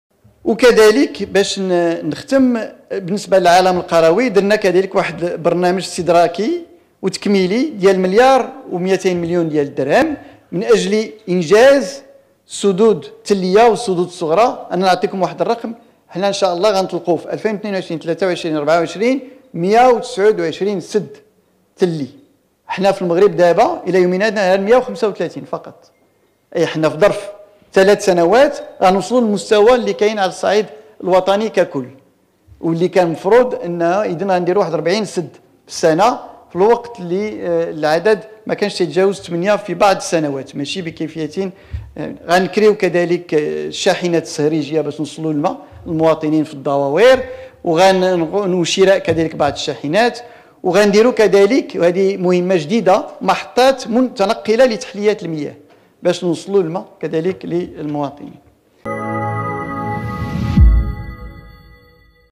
كلمة السيد نزار بركة، وزير التجهيز والماء حول موضوع الماء كرهان مغربي
كلمة السيد نزار بركة، وزير التجهيز والماء، خلال استضافته يوم الخميس 03 مارس 2022 من قبل نادي الصحافة بالمغرب بشراكة مع المعهد العالي للإعلام والاتصال، حول موضوع "الماء كرهان مغربي" حيث جدد تأكيده على أنه :
"بالنسبة للعالم القروي، عملنا على إطلاق برنامج تكميلي بغلاف مالي قدره مليار و200 مليون درهم لإنجاز السدود التلية والصغرى، كما أنه وفي ظرف ثلاث سنوات سيتم إطلاق 129 سدا تليا، كما سيتم كراء شاحنات صهريجية، وشراء شاحنات صهريجية، وتثبيت محطات متنقلة لتحلية المياه".